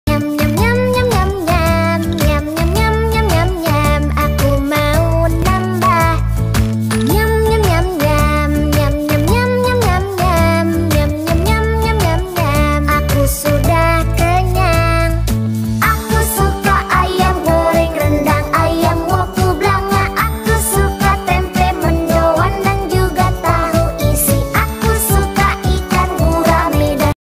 CAMILAN DARI TEMPE YANG BELUM JADI CHANEL BUNDA SYAPNIAR
BATEMGOR ( bakso Tempe goreng )
dari tempe yg belum jd fermentasi .